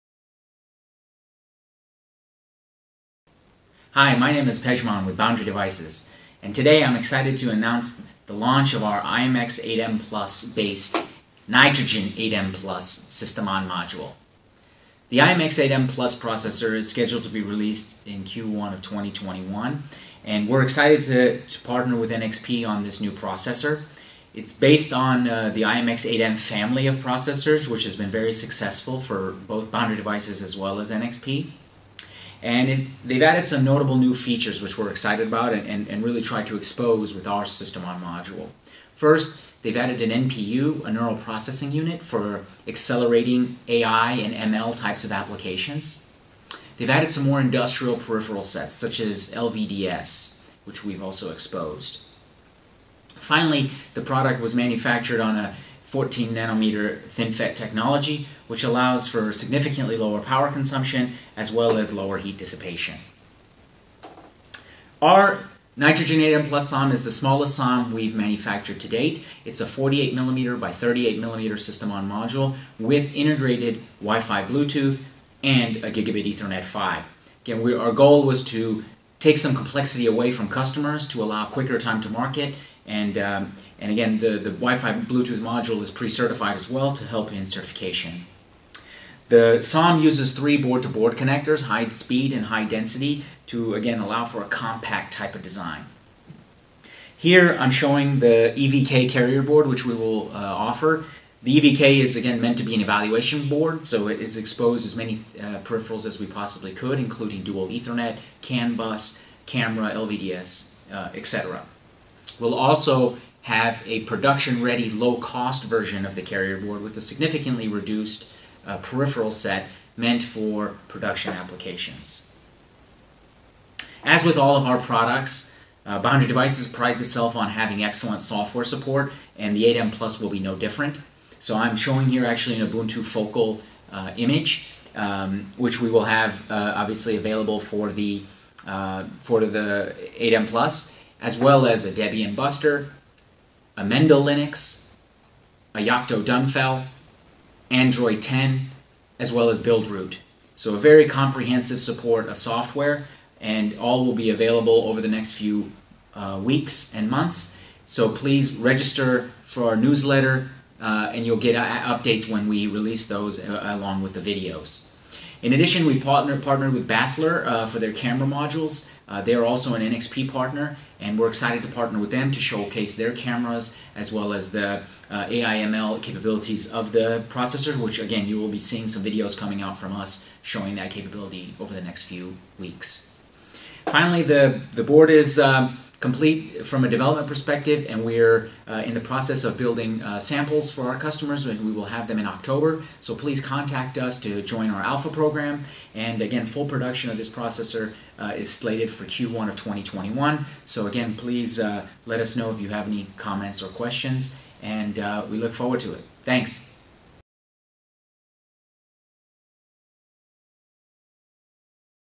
Nitrogen8M Plus SOM - i.MX 8M Plus System on Module
Take a look at the newest System on Module in the Nitrogen8M Family from Boundary Devices!  The i.MX8M PLUS based System on Module is an exciting new release from Boundary Devices.  The i.MX8M PLUS has some notable features including:
- LVDS, HDMI and MIPI DSI support for multiple display applications
- Built-in Neural Processing Unit (NPU) for real time object detection and machine learning
- Integrated CAN bus for industrial applications
- The Nitrogen8M PLUS is 48mm x 38mm and includes on-board Ethernet PHY and WiFi+BT.